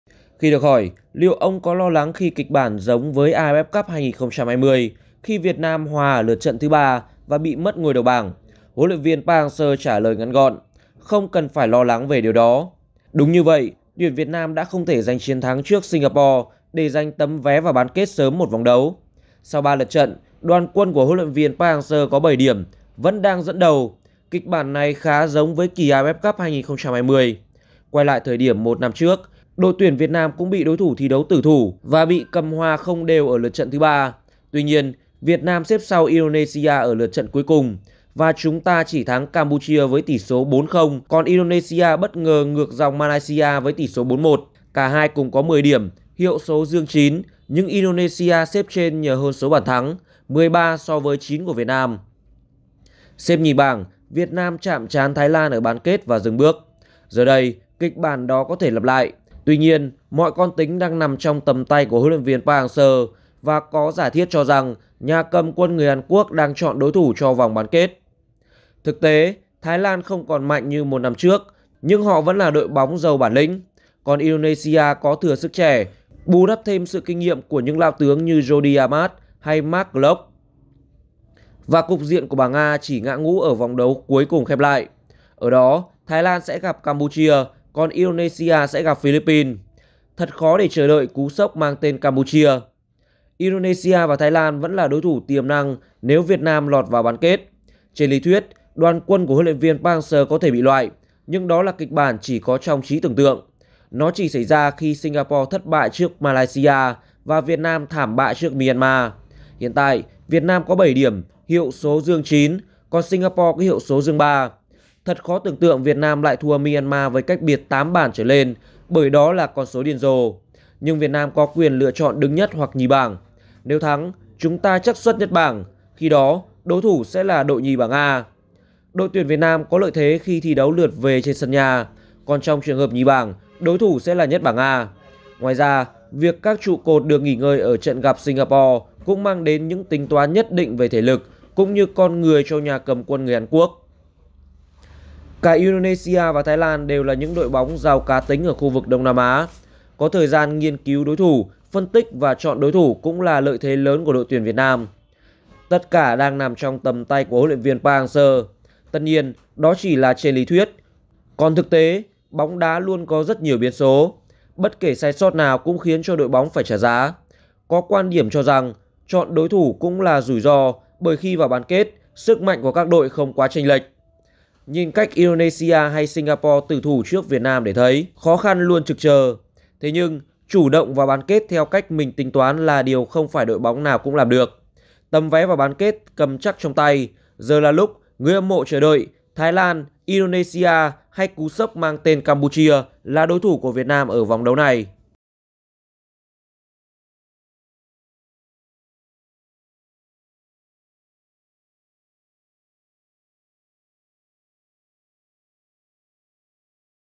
HLV Park Hang Seo ủ mưu: Chọn Thái Lan hay Indonesia ở bán kết AFF Cup 2022?
Tuyển Việt Nam cầm chắc tấm vé vào bán kết. Giờ là lúc, HLV Park Hang Seo toan tính cho đối thủ ở vòng đấu này, đó có thể là Thái Lan hay Indonesia hoặc bất ngờ hơn nữa là Campuchia.